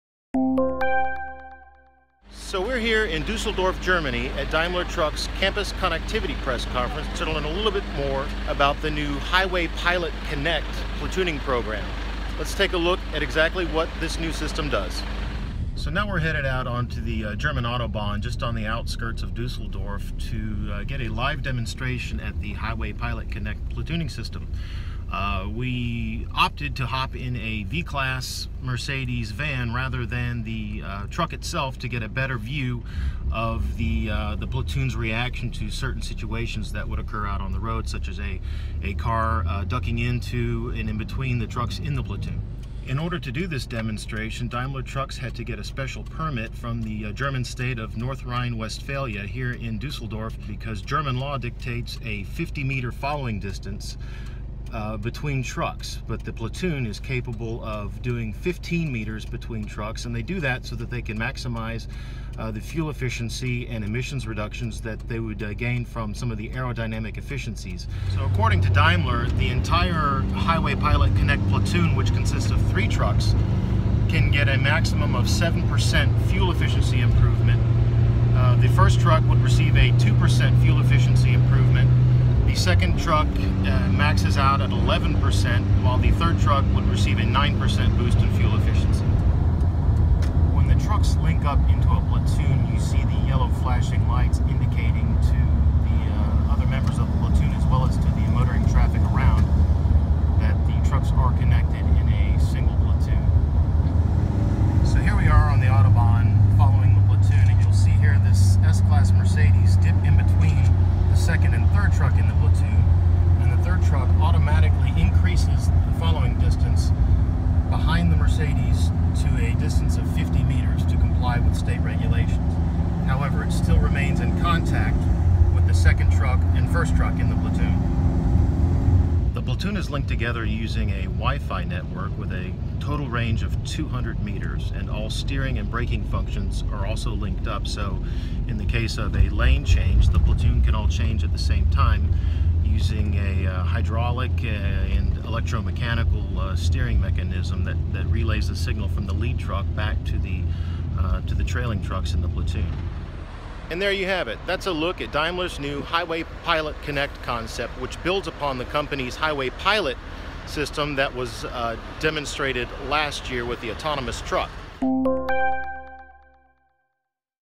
Daimler Trucks Highway Pilot Connect: Truck Platooning
In an on-highway demonstration on the Autobahn on the outskirts of Dusseldorf, Daimler showcases its new autonomous capabilities with a platoon of three Highway Pilot Connect-equipped Mercedes Actros trucks.